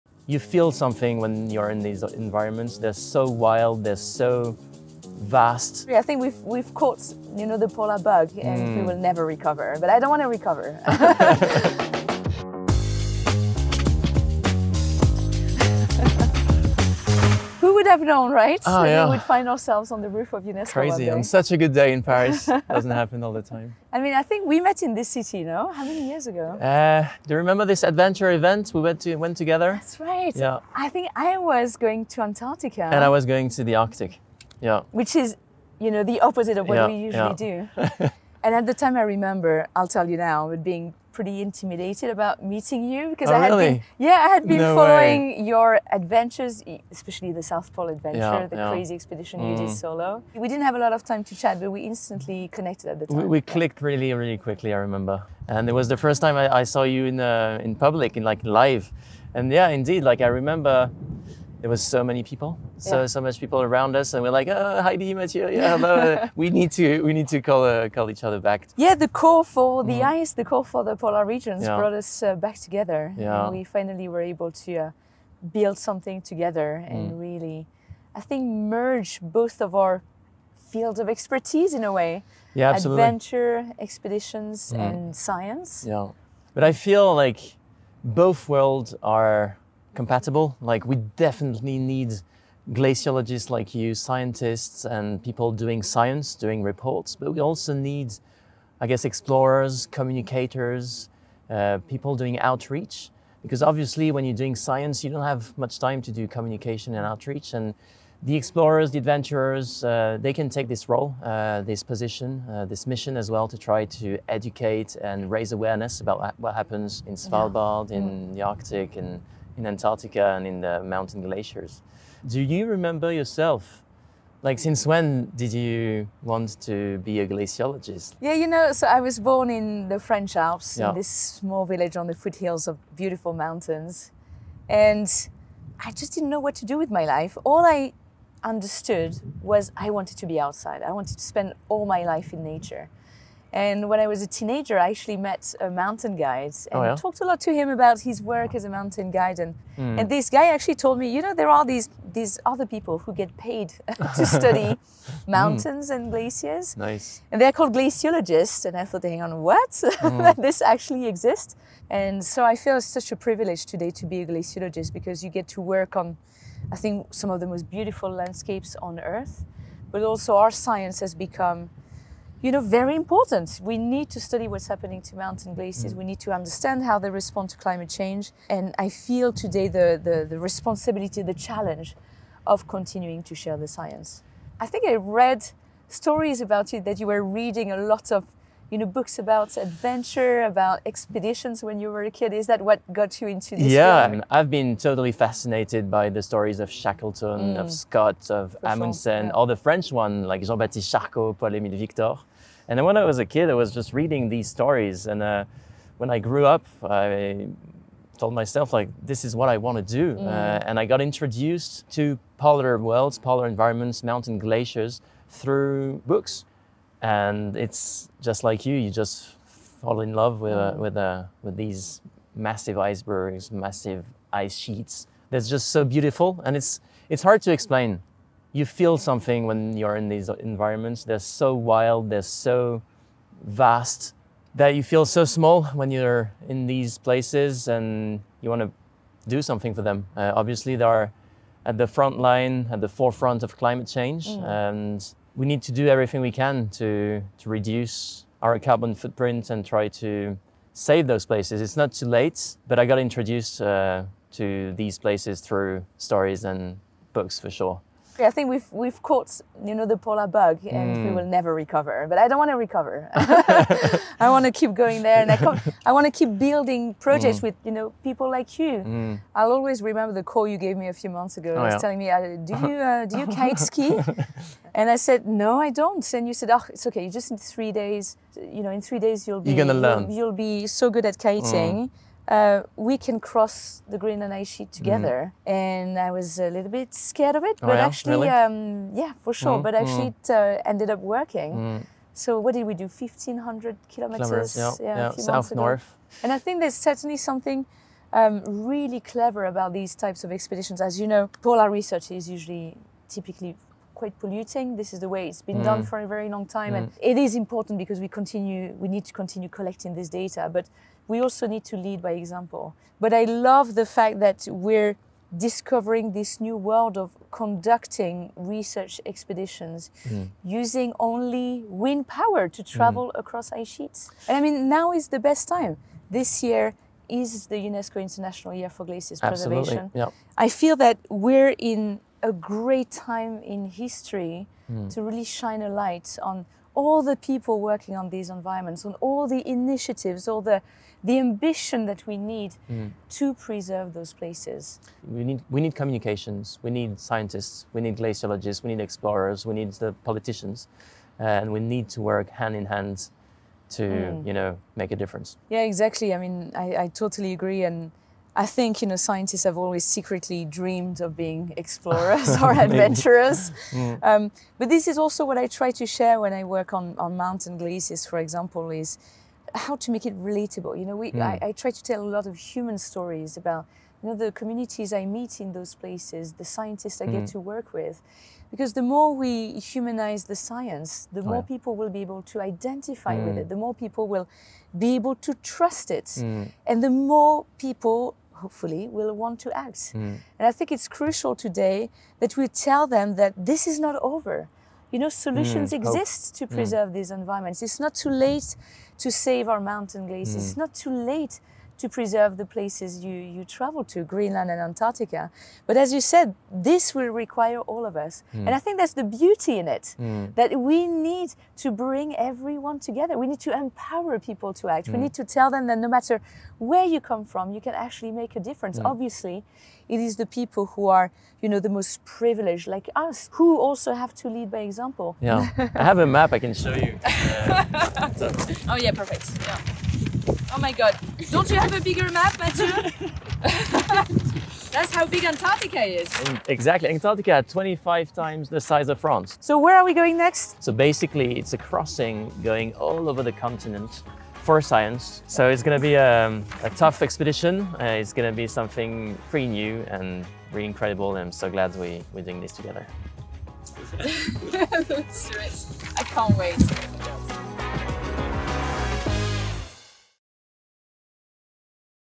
Explorer to Glaciologist: What does it take to protect our glaciers?
"You feel something out there—it’s wild, vast, humbling. And you just want to protect it."

From solo expeditions across Antarctica to mountain science in the French Alps, Heïdi and Mathieu bring together two worlds: adventure and science. In this moving conversation on UNESCO’s rooftop, they share how books, ice, and bold dreams shaped their paths—and how climate action needs us all.

As the International Year of Glaciers’ Preservation unfolds, their message is clear: it’s not too late to act. But we must act together.

What would you do to protect the polar regions?